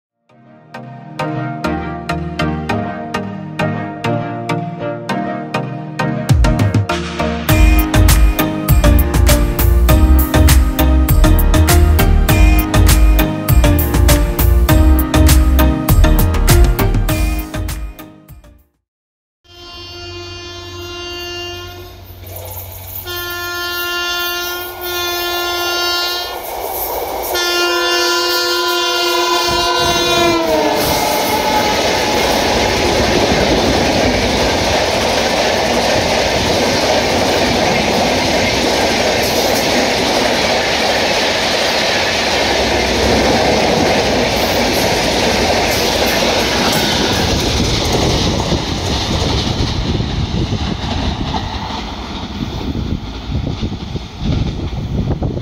Acknowledgment by loco pilot | Kochuveli–Amritsar Weekly Express at tughlkabad
Camera Used
Cannon SX70

indian railways video on youtube

Rail Travelers